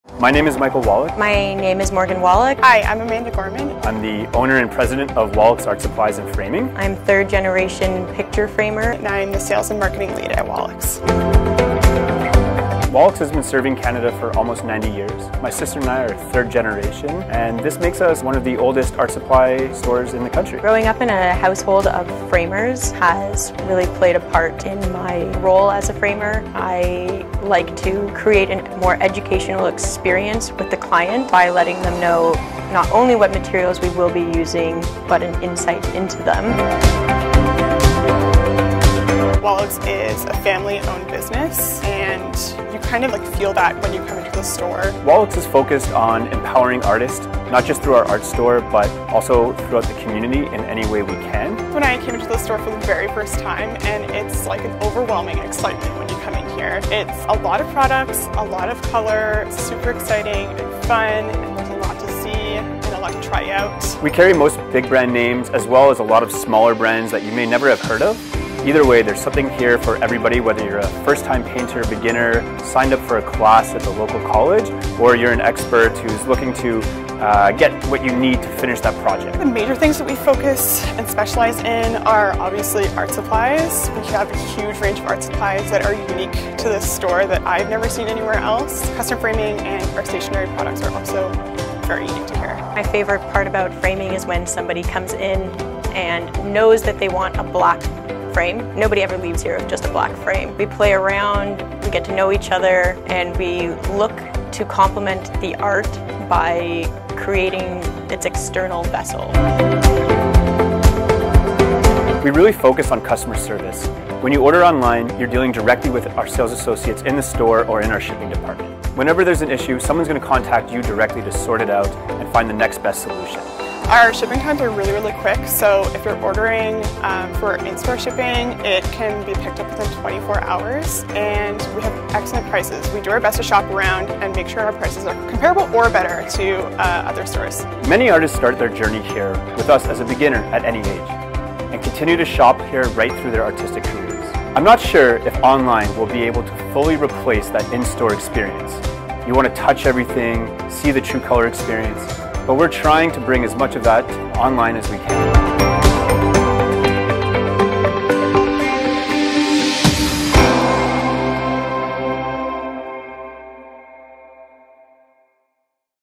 Wallack's Family Business Introduction
With more artists moving to order their supplies online during the Ontario pandemic lockdowns, we wanted to share a little video about who we are and who you are ordering your supplies from.

Wallack's Art Supplies and Framing has been serving the Ottawa region and across Canada for 90 years. Today, we offer a great selection of artist's materials at competitive prices. Our services and turnaround time on orders are amazingly fast and we are always here to help.